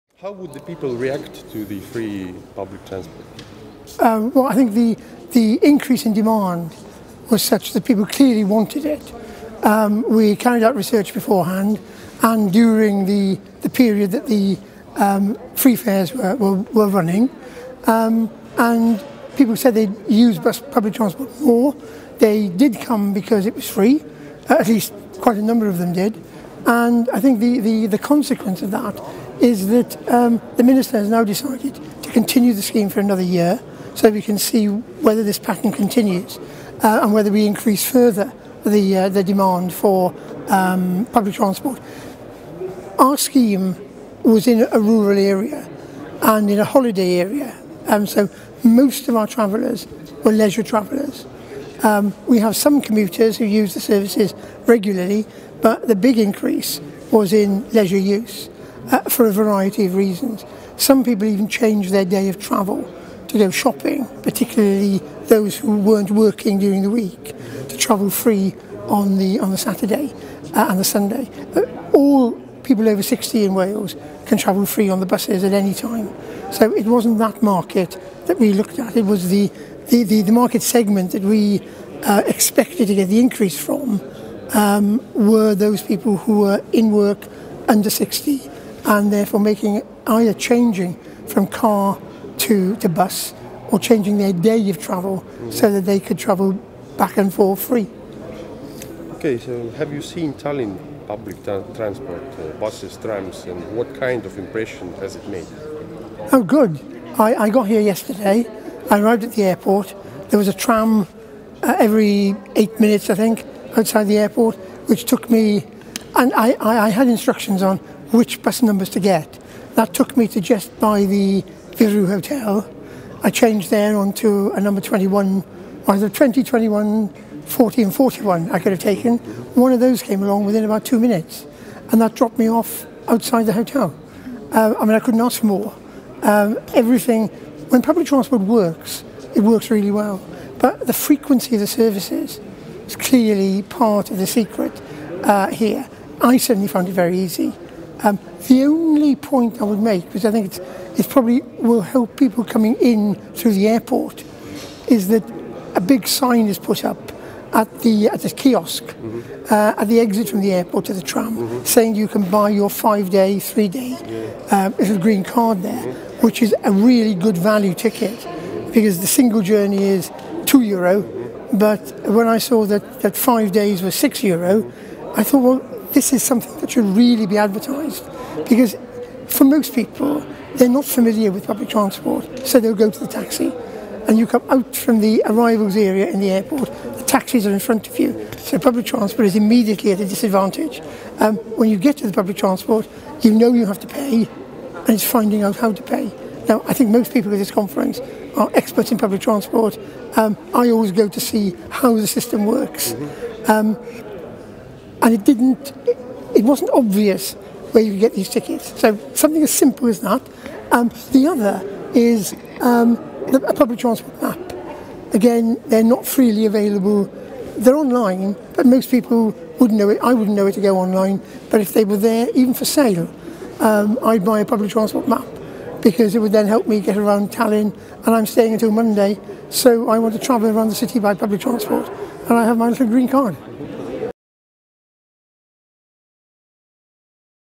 Professor Stuart Cole: Tallinna edu saladus peitub tihedas sõidugraafikus
„Meil on praegu selline süsteem, et laseme inimestele tasuta sõita laupäeviti, kui käiakse rohkem poes ja sõpradel külas,“ ütles Lõuna-Walesi ülikooli professor Stuart Cole.